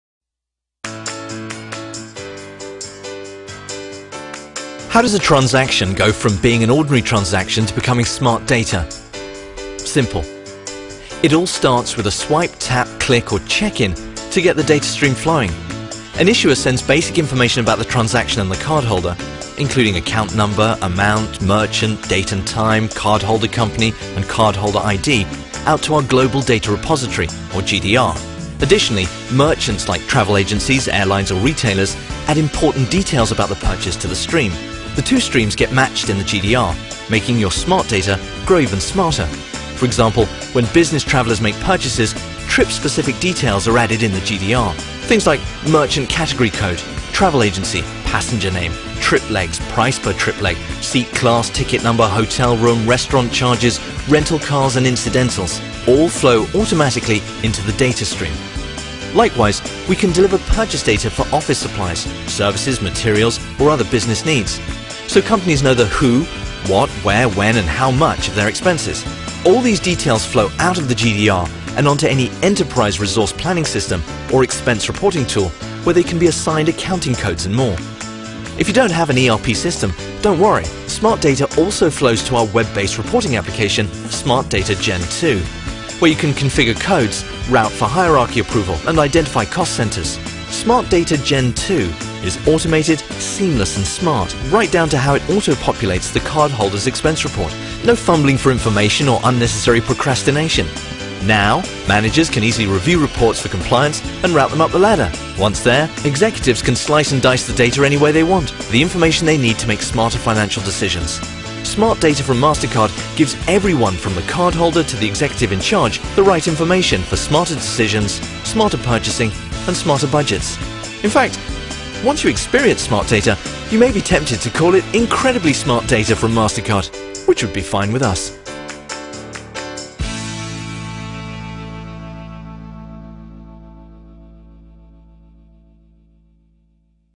Mastercard® Smart Data
The Mastercard Smart Data suite delivers global, web-based reporting to help you seamlessly organize, consolidate, analyze and manage rich financial data. See how Mastercard Smart Data can help you analyze and manage rich financial data.